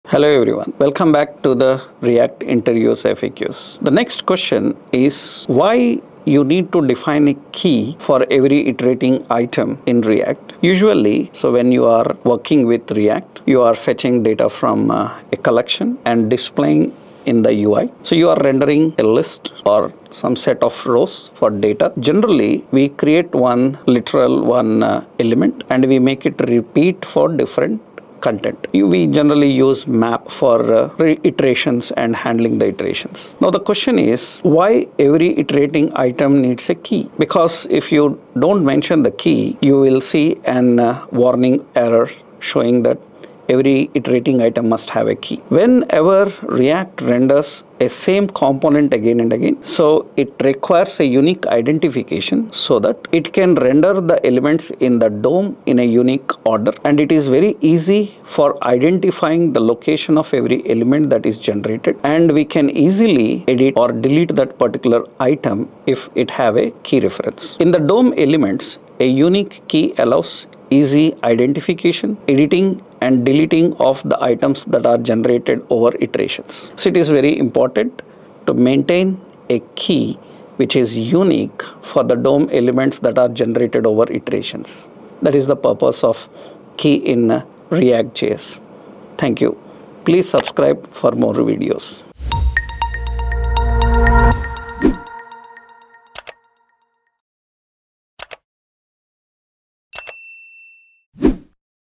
Why is ‘key’ Required in React Iterations? | ReactJS Interview FAQ Explained | Naresh IT
👋 Welcome to React Interview FAQs by Naresh i Technologies!
In this session, we answer a frequently asked frontend interview question:
🧠 "Why do we need to define a key for every iterating item in React?"

🎯 You’ll Learn:
✅ What happens when rendering lists with .map()
✅ Why React throws warnings without key
✅ How keys help React identify, update & delete elements efficiently
✅ Best practices for assigning unique keys
✅ Performance and re-rendering improvements using key

📌 Pro Tip: Always use a stable and unique key — avoid using array indexes in dynamic lists!

🔁 Perfect explanation for freshers and working professionals preparing for React interviews.

💬 Drop your React questions in the comments — we’ll answer them in the next video!